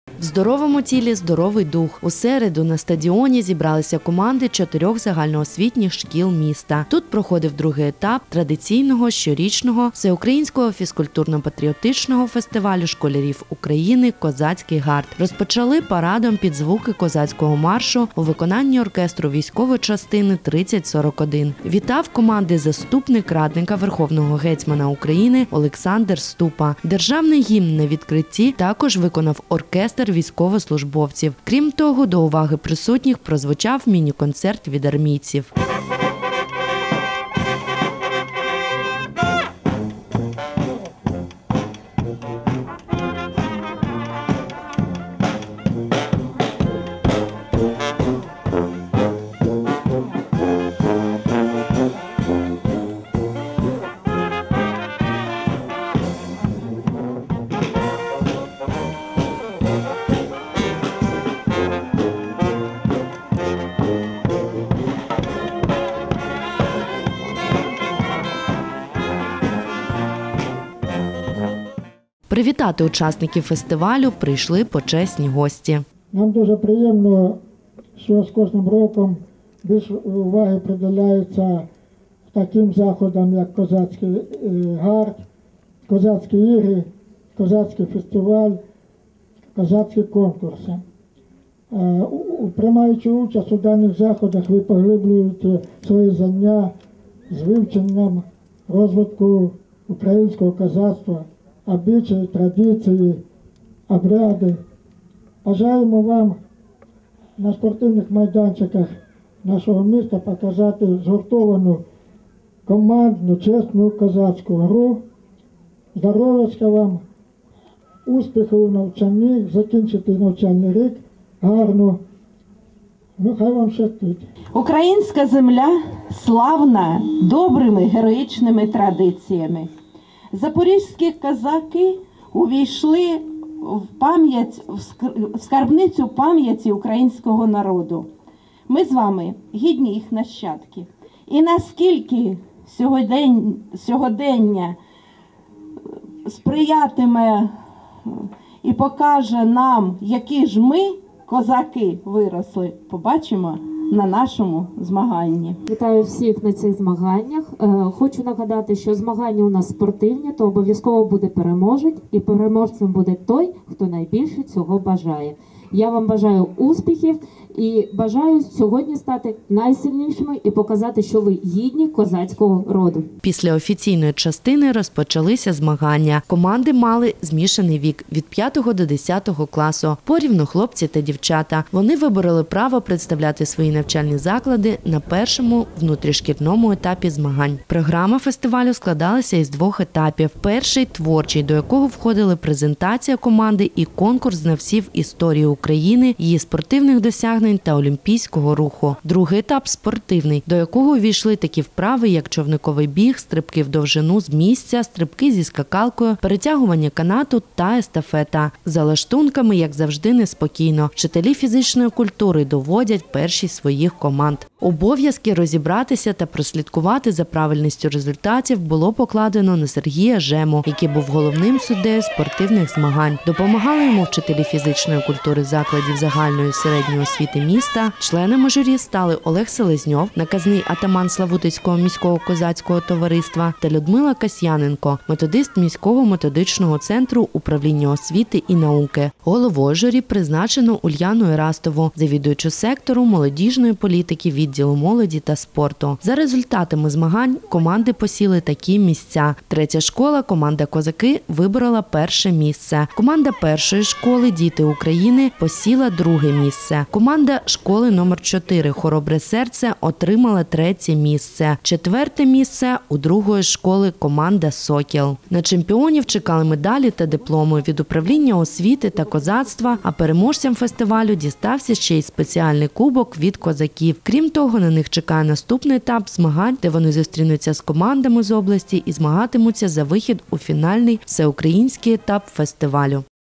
Козацький гарт - 2019
На міському стадіоні «Каскад» відбувся міський етап Всеукраїнського фізкультурно-патріотичного фестивалю школярів України «Козацький гарт - 2019»

Допомогали в підготовці та проведенні заходу: КП УФКСТ (Філіппов С.В.), відділу молоді та спорту виконавчого комітету Славутицької міької ради (Шаніна Л.М.), Славутицькому міському козацькому товариству (Яворський О.М.), в/ч 3041 Національної гвардії України (Чеботарь В.І.), Славутицьким ЗОШ І-ІІІ ступенів №1, 2 (Бардакова Н.О., Ігнатова А.В.). та Славутицькому ліцею (Нечай Т.О.)

Почесну перемогу виборола команда Славутицької ЗОШ І-ІІІ ступенів №3, ІІ місце - команда Славутицької ЗОШ І-ІІІ ступенів №1, ІІІ місце посіла команда Славутицької ЗОШ І-ІІІ ступенів №4.